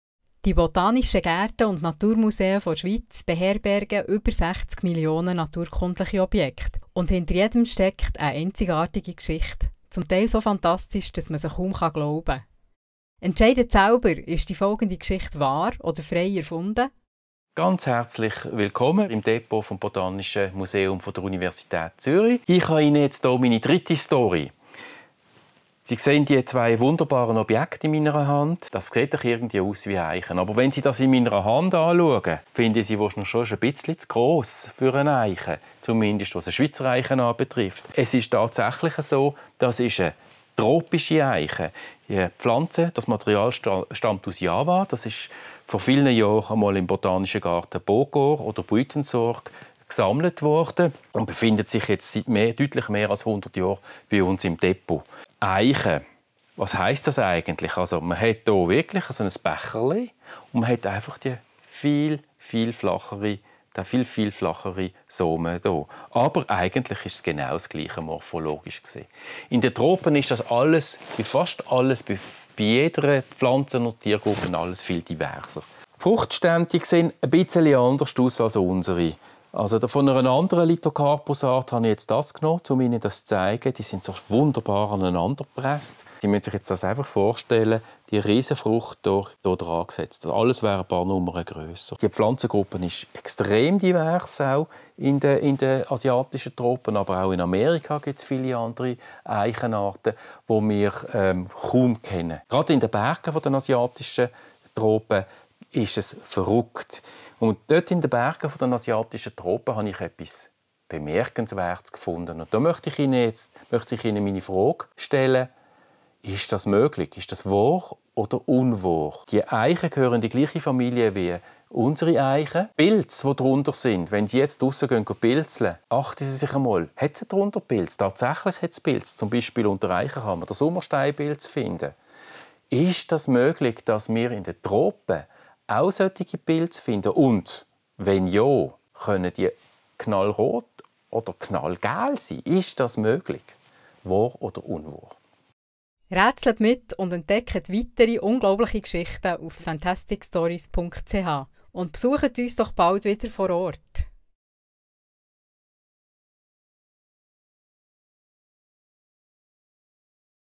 Pilze unter Eichen auf Java
Am nationalen Tag der naturhistorischen Sammlungen bieten über 20 Institutionen aus der ganzen Schweiz einmalige Einblicke hinter die Kulissen. Und bereits im Vorfeld erzählen Kurator*innen Geschichten zu ihren Sammlungsobjekten.

Aber Achtung: Ist die Geschichte von Kurator Alexander Kocyan aus dem Botanischen Museum der Universität Zürich wahr oder frei erfunden? Zu gewinnen gibt es einmalige Preise.